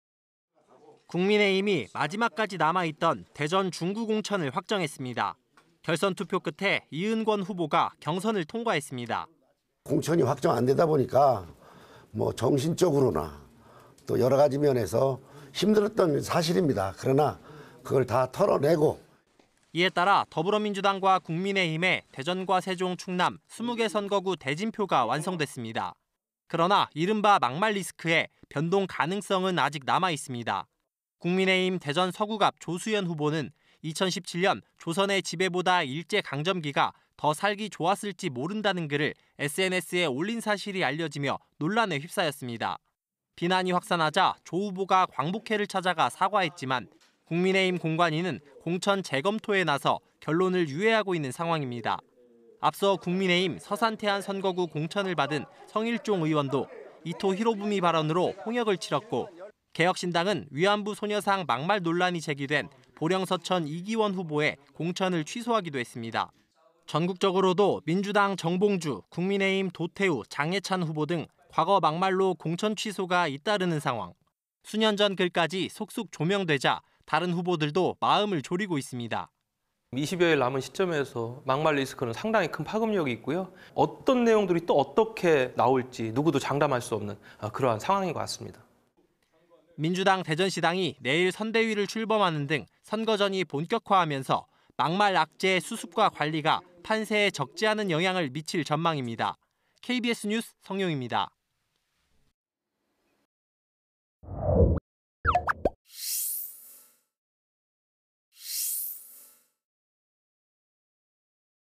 [총선] 민주당·국민의힘 대진표 완성…‘막말 리스크’ 막판 변수 / KBS 2024.03.18.
[리포트]

 국민의힘이 마지막까지 남아 있던 대전 중구 공천을 확정했습니다.

 결선 투표 끝에 이은권 후보가 경선을 통과했습니다.

 [이은권/국민의힘 예비후보 : "공천이 확정 안 되다 보니까 정신적으로나 여러 가지 면에서 힘들었던 게 사실입니다. 그러나 그것을 다 털어 내고..."]

 이에 따라 더불어민주당과 국민의힘의 대전과 세종, 충남 20개 선거구 대진표가 완성됐습니다.

 그러나 이른바 '막말 리스크'에 변동 가능성은 아직 남아 있습니다.

 국민의힘 대전 서구갑 조수연 후보는 2017년 '조선의 지배보다 일제강점기가 더 살기 좋았을지 모른다'는 글을 SNS에 올린 사실이 알려지며 논란에 휩싸였습니다.

 비난이 확산하자  조 후보가 광복회를 찾아가 사과했지만, 국민의힘 공관위는 공천 재검토에 나서  결론을 유예하고 있는 상황입니다.

 앞서 국민의힘 서산·태안 선거구 공천을 받은 성일종 의원도 이토 히로부미 발언으로 홍역을 치렀고, 개혁신당은 '위안부 소녀상 막말' 논란이 제기된 보령·서천 이기원 후보의 공천을 취소하기도 했습니다.

 전국적으로도 민주당 정봉주, 국민의힘 도태우, 장예찬 후보 등 과거 막말로 공천 취소가 잇따르는 상황.

 수년 전 글까지 속속 조명되자 다른 후보들도 마음을 졸이고 있습니다.

 [이희성/단국대 정책경영대학원 교수 : "20여 일 남은 시점에서 막말 리스크는 상당히 큰 파급력이 있고요. 어떤 내용들이 또 어떻게 나올지 누구도 장담할 수 없는 그런 상황인 것 같습니다."]

 민주당 대전시당이 내일 선대위를 출범하는 등 선거전이 본격화되면서 '막말 악재' 수습과 관리가 판세에 적지 않은 영향을 미칠 전망입니다.

 KBS 뉴스 성용희입니다.

 촬영기자:강수헌